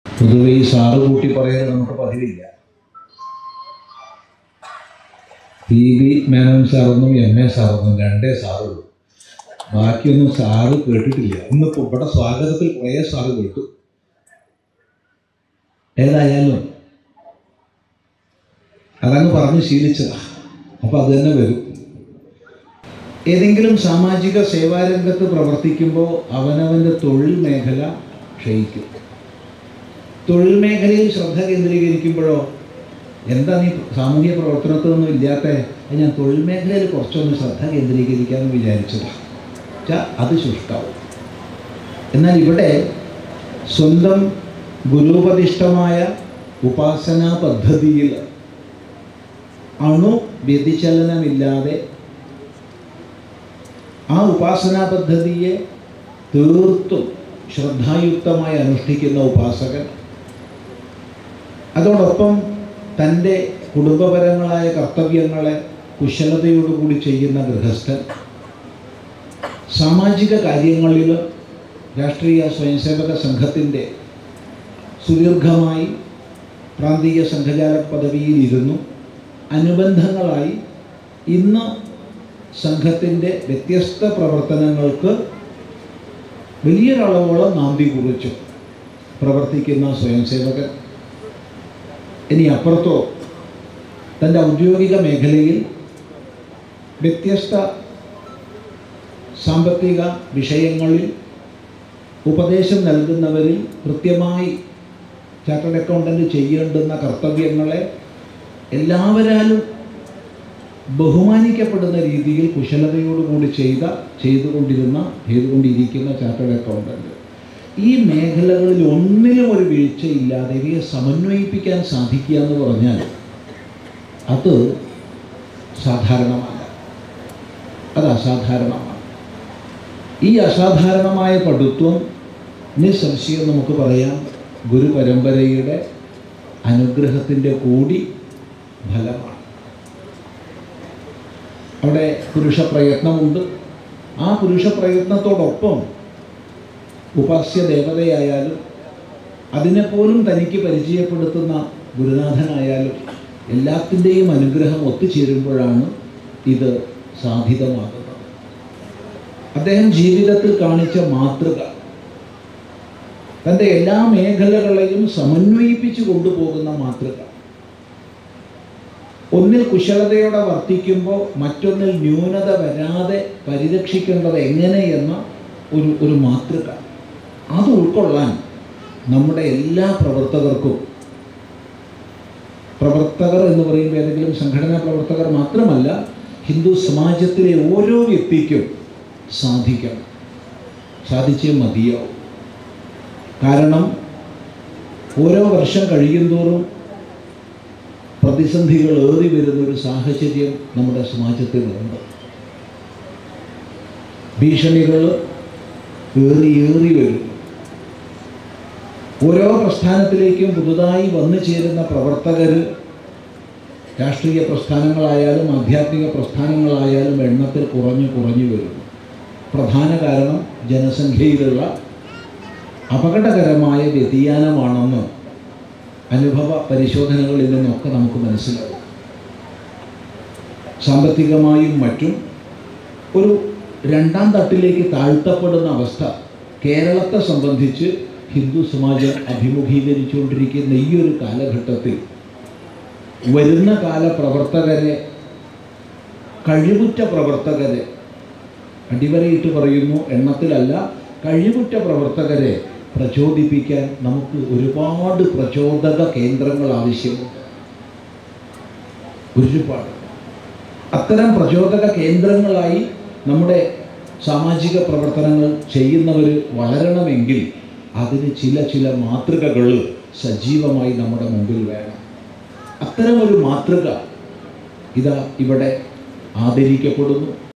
രണ്ടേ സാറുള്ളൂ, മേനോൻ സാറും എംഎ സാറും,പിഇബി മേനോന്റെ ശതാഭിഷേക ആഘോഷചടങ്ങിൽ ചിദാനന്ദപുരി സ്വാമികൾ
രണ്ട് സാറൻമാരേ ഉളളൂ, മേനോൻ സാറും എംഎ സാറും,പിഇബി മേനോന്റെ ശതാഭിഷേക ആഘോഷചടങ്ങിൽ ചിദാനന്ദപുരി സ്വാമികൾ